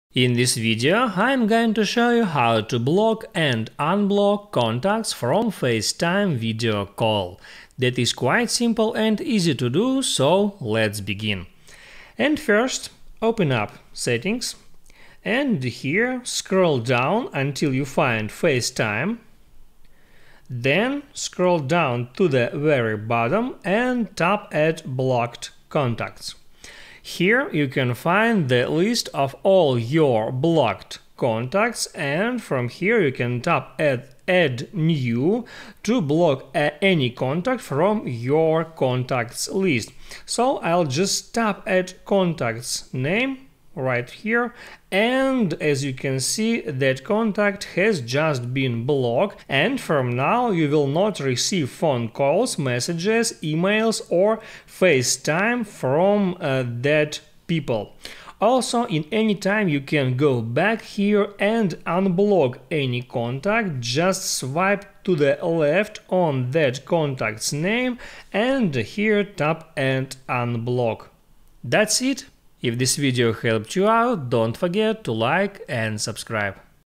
How to Block & Unblock Contacts on FaceTime Call
In this video i show simple way How to Block & Unblock Contacts on FaceTime Call